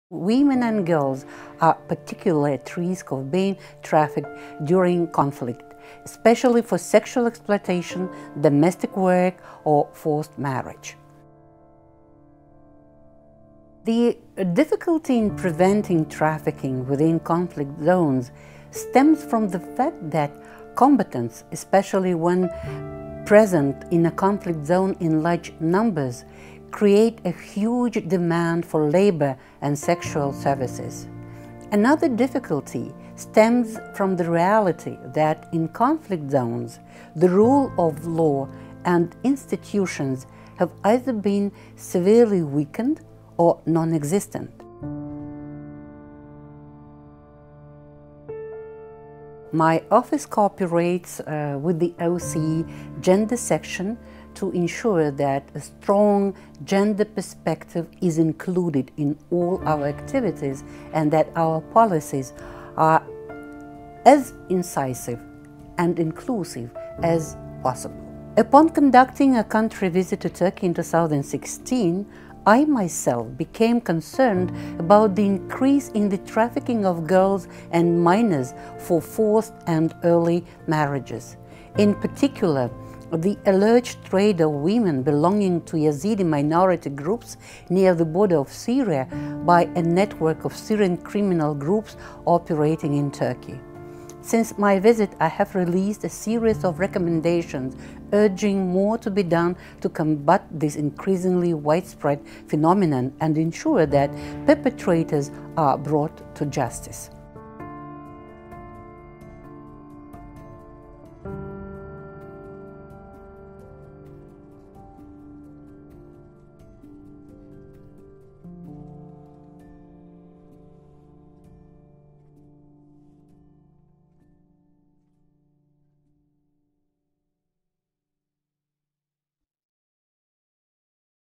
OSCE Anti-Trafficking Representative Madina Jarbussynova
Ambassador Madina Jarbussynova of Kazakhstan is the OSCE Special Representative and Co-ordinator for Combating Trafficking in Human Beings. She discusses how women and girls are at increased risks of human trafficking during times of conflict. Through her work with the OSCE, Ambassador Jarbussynova works to prevent labor exploitation and trafficking in conflict zones in Europe and Asia.